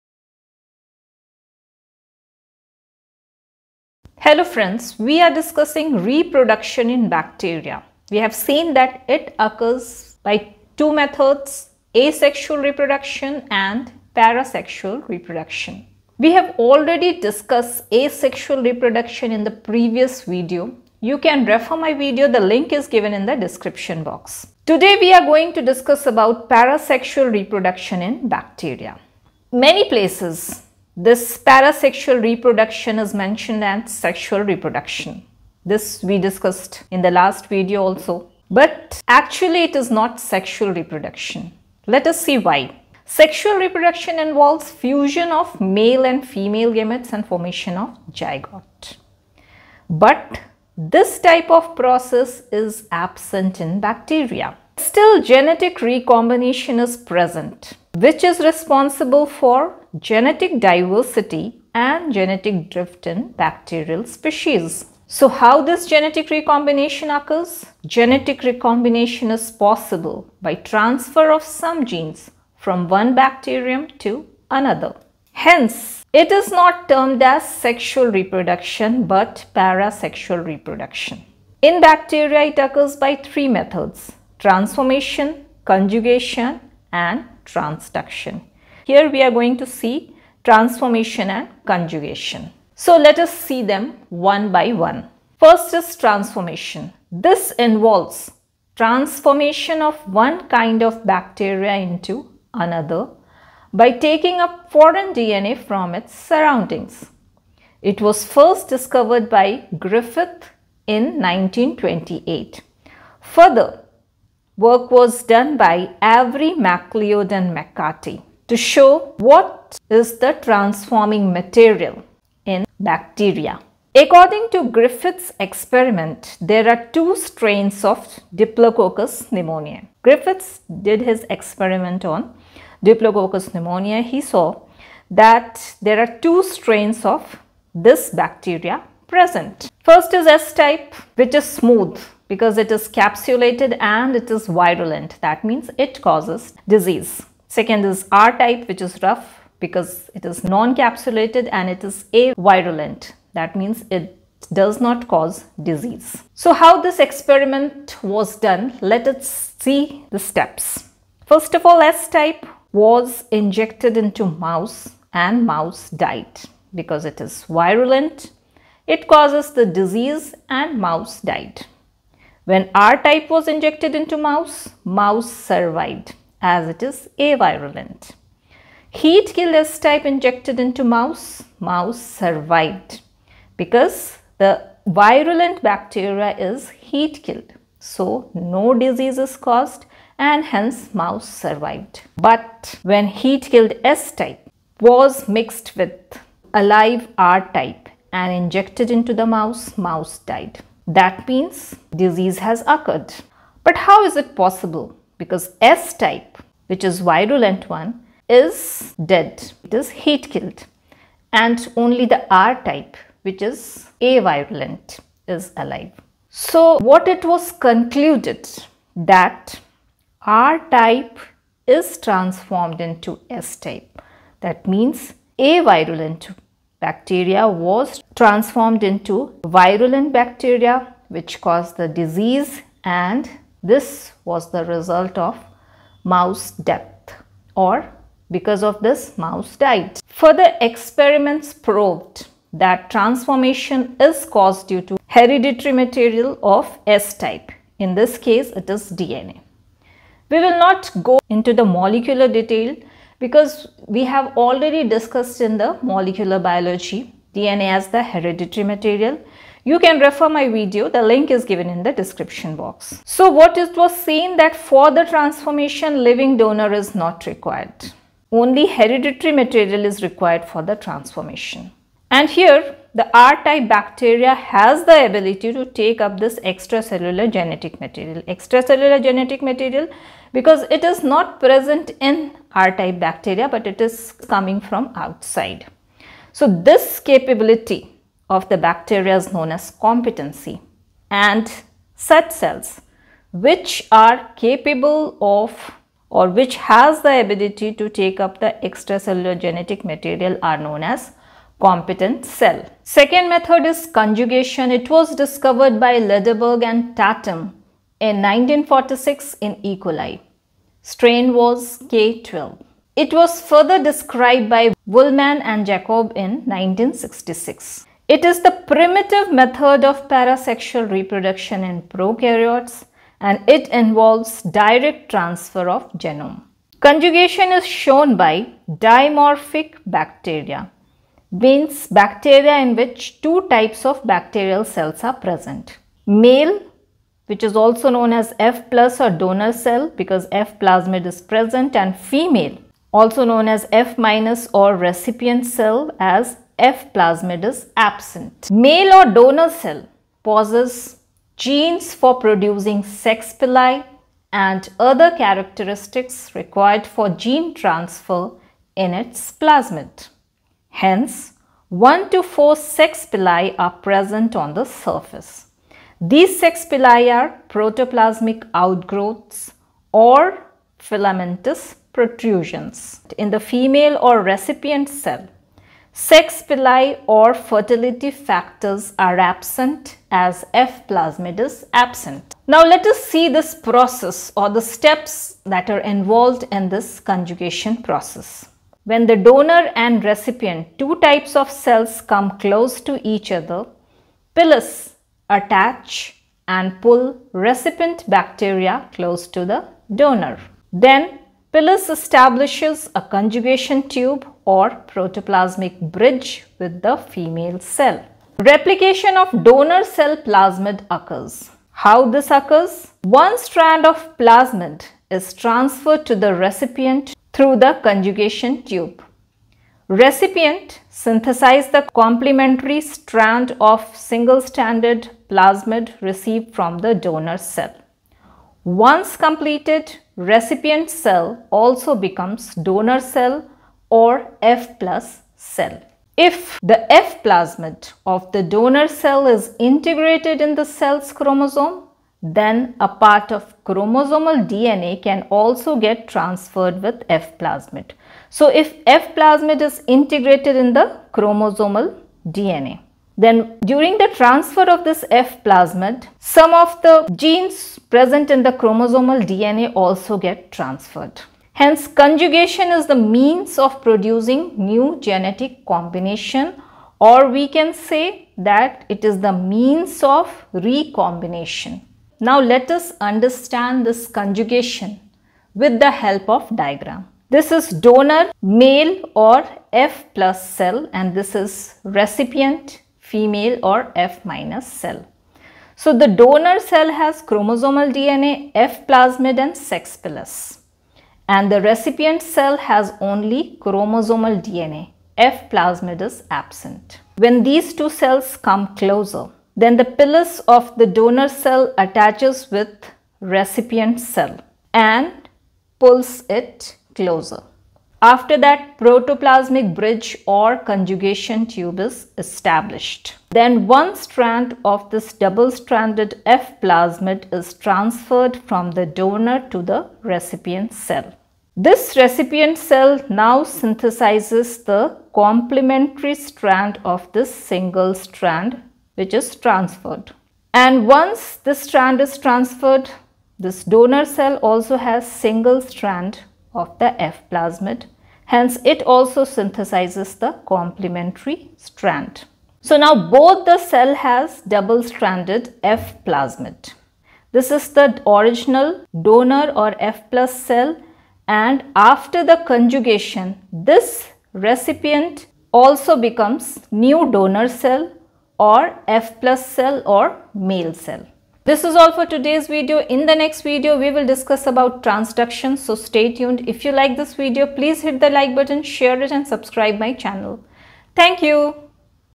Transformation and Conjugation in bacteria. For NEET Biology and CSIR NET.
In this video Parasexual Reproduction in bacteria is explained .Many places it is mentioned as sexual Reproduction, here it is discussed why it is called Parasexual Reproduction and not sexual Reproduction. It occurs by 3 methods. 2 methods Transformation and Conjugation are explained in detail step by step with the help of diagram.

Parasexual Reproduction is also available in HINDI, the link is